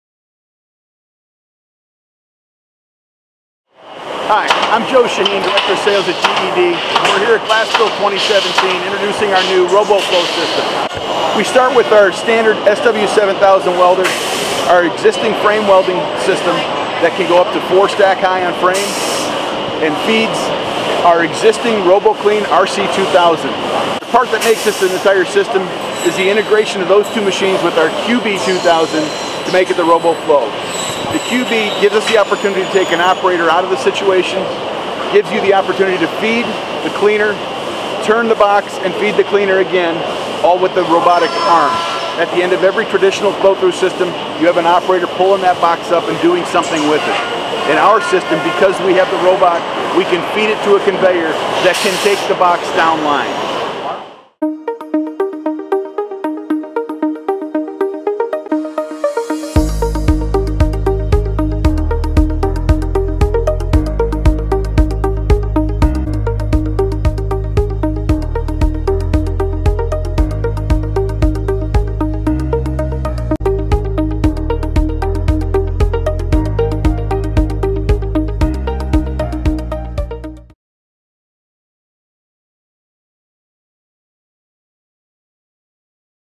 GED's RoboFlow™ QB Automated Window System at GlassBuild America 2017
This video highlights features of our new RoboFlow Window Frame System - Welder - QB Materials System - Cleaner.